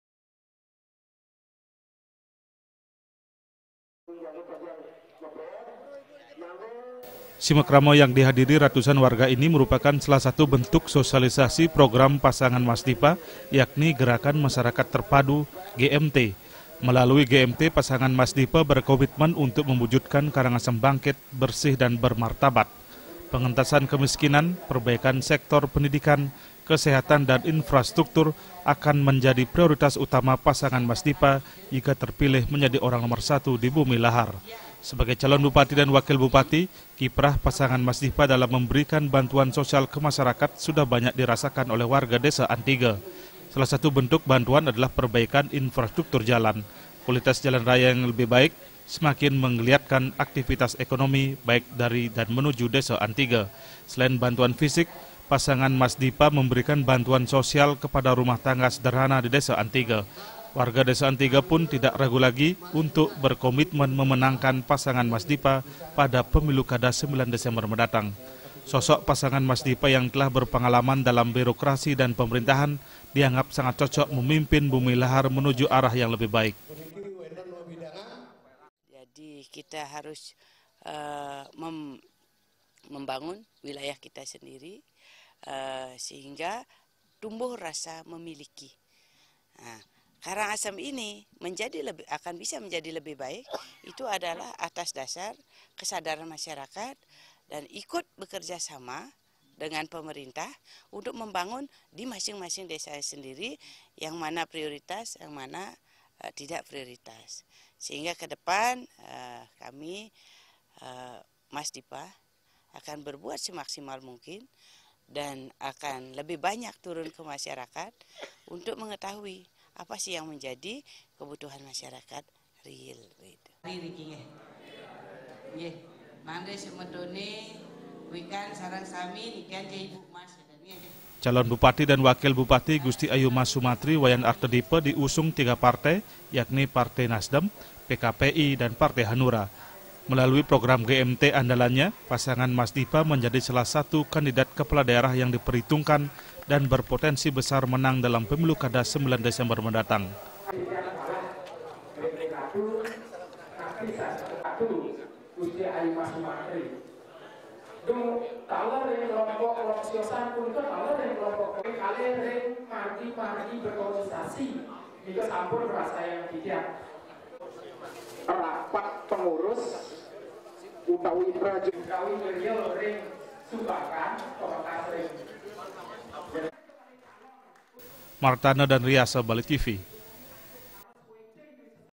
DESA ANTIGA KOMIT MENANGKAN MASDIPA
CALON BUPATI DAN WAKIL BUPATI KARANGASEM, GUSTI AYU MAS SUMATRI - WAYAN ARTHA DIPA, MENGGELAR SIMAKRAMA DI DESA ANTIGA KECAMATAN MANGGIS. WARGA PUN SANGAT ANTUSIAS MENGIKUTI SIMAKRAMA, DAN SIAP MEMENANGKAN PASANGAN MASDIPA PADA PEMILUKADA 9 DESEMBER MENDATANG.